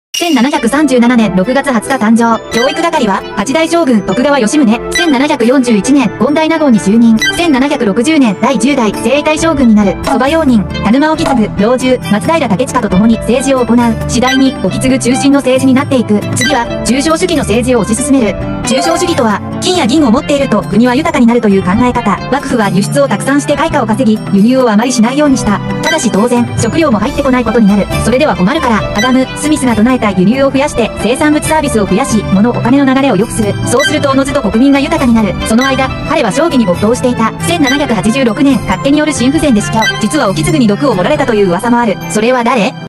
6/20 歴史クイズ21～彼は誰？～ 徳川吉宗 田沼意次 アダム・スミス
彼なりに、最初は、将軍職を全うしようと努力したが、部下にも恵まれたが、なかなか政治がうまくいかず、最後には匙を投げてしまった。